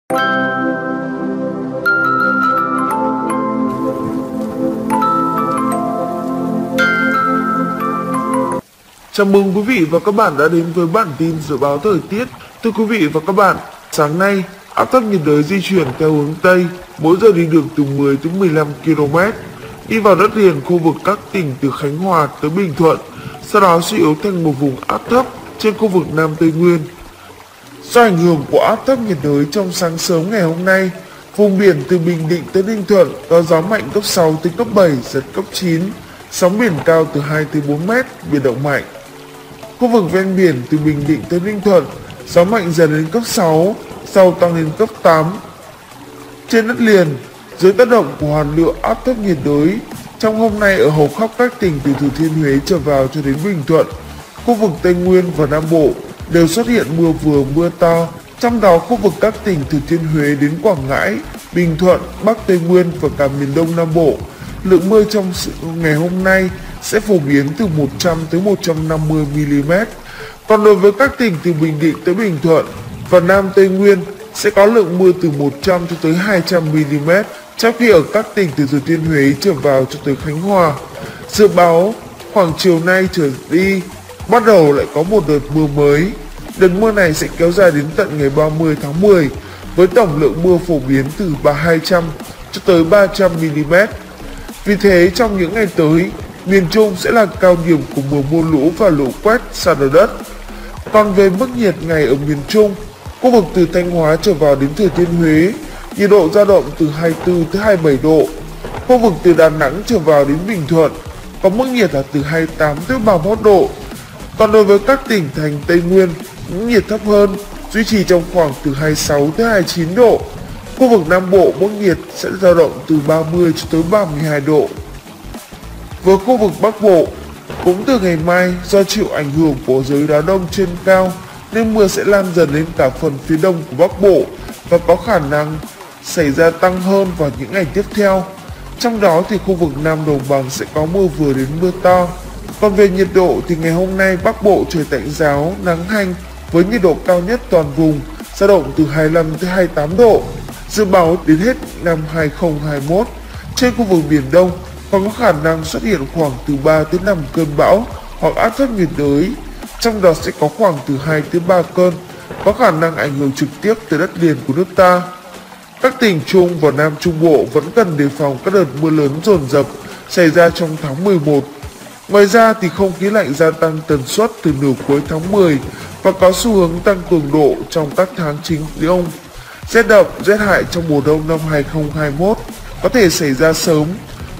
Dự báo thời tiết hôm nay, ngày mai 28 tháng 10 năm 2021 / Tin Bão /Thời tiết 3 ngày
Weather forecast for today, tomorrow October 28, 2021 / Storm News /3-day weather